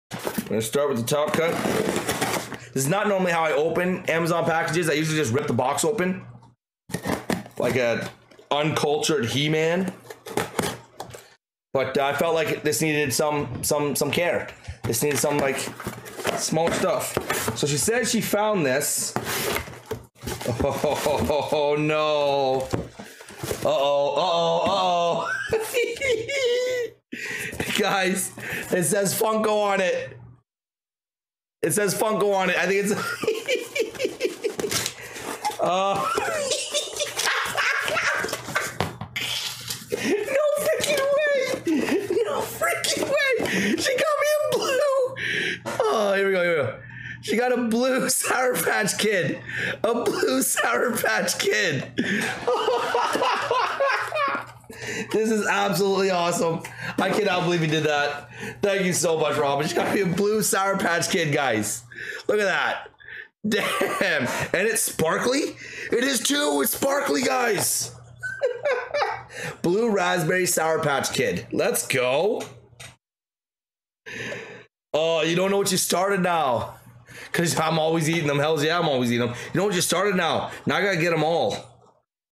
A surprise in the mail
A little gift from a community member and mod to the channel. Thank you all for the amazing support.